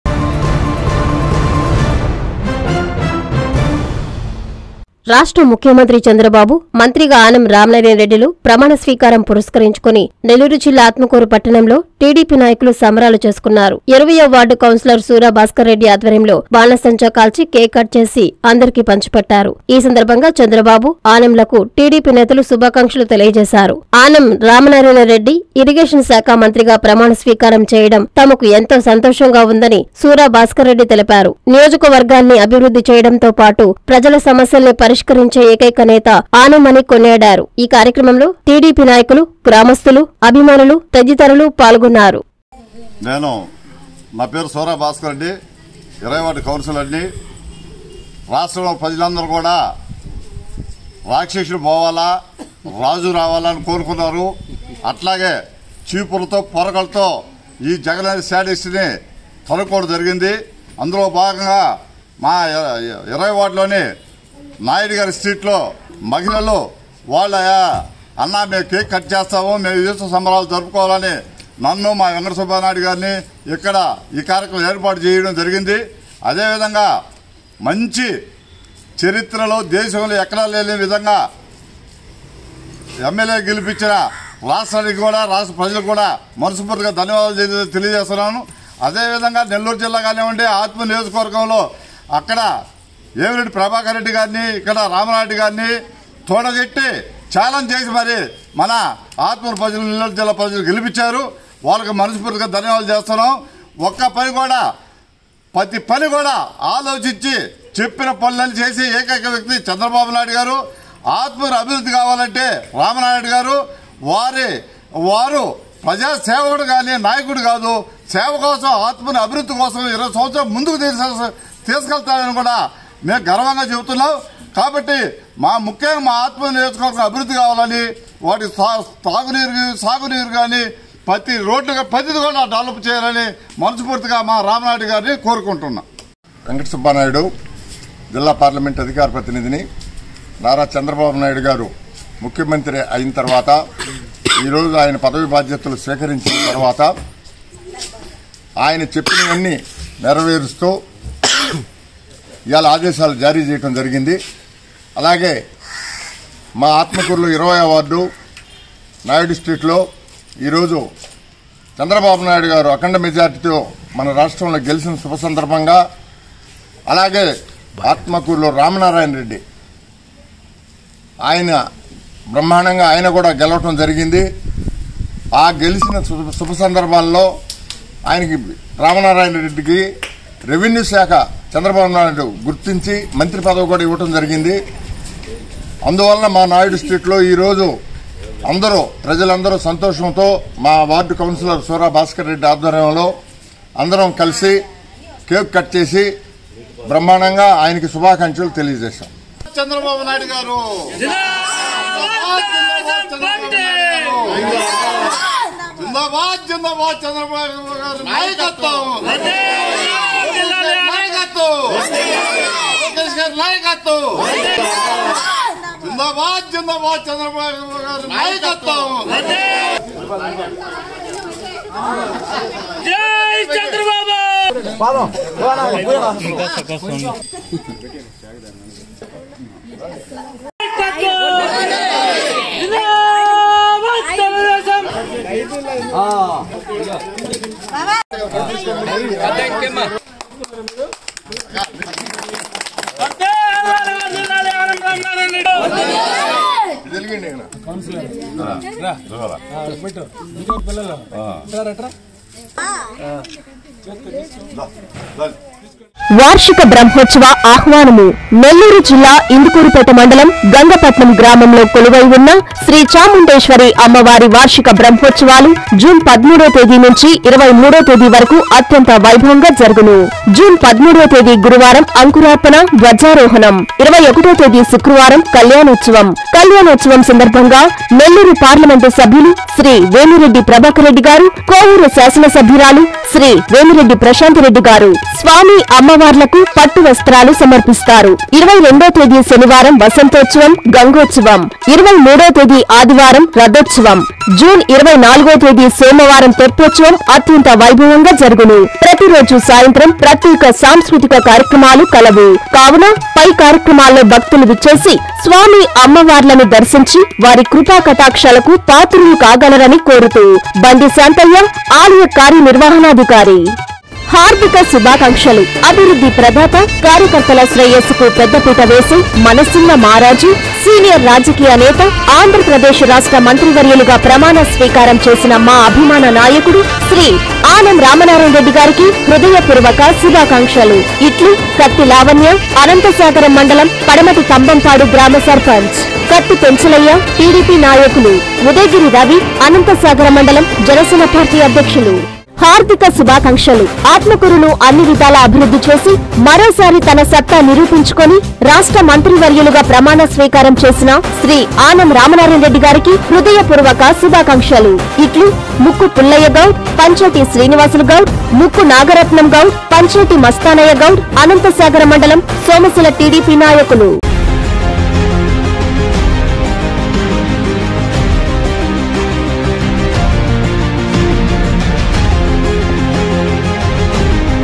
ఆత్మ‌కూరులో టీడీపీ సంబ‌రాలు
- సూరా భాస్క‌ర్ రెడ్డి ఆధ్వ‌ర్యంలో కేక్ క‌టింగ్